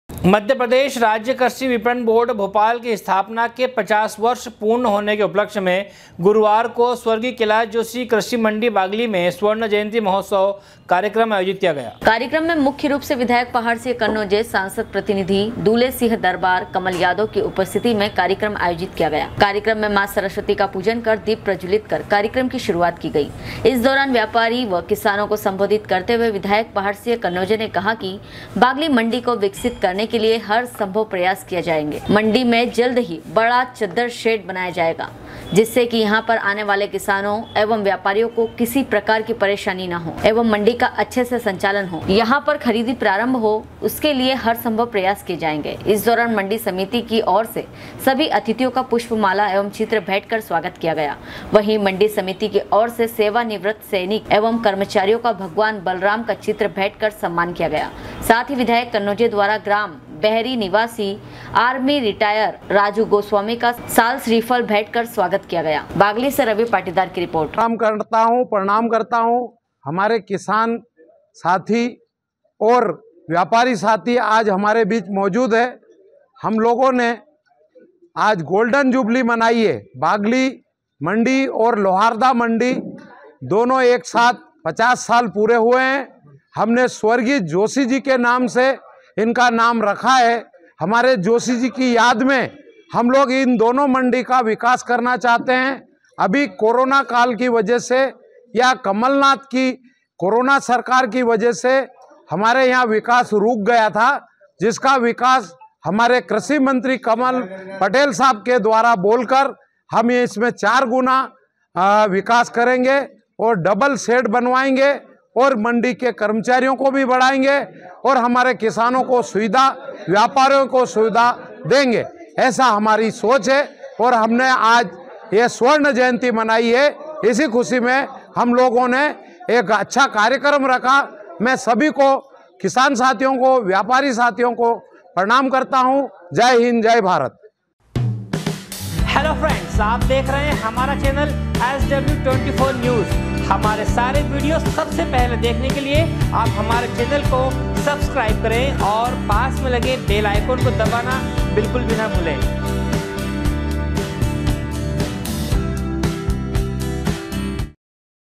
मध्यप्रदेश राज्य कृषि विपनान बोर्ड भोपाल की स्थापना के 50 वर्ष पूर्ण होने के उपलक्ष्य में गुरुवार को
मध्यप्रदेश राज्य कृषि विपनान बोर्ड भोपाल की स्थापना के 50 वर्ष पूर्ण होने के उपलक्ष्य में गुरुवार को स्व. कैलाश जोशी कृषि मंडी बागली में स्वर्ण जयंती महोत्सव कार्यक्रम आयोजित किया गया।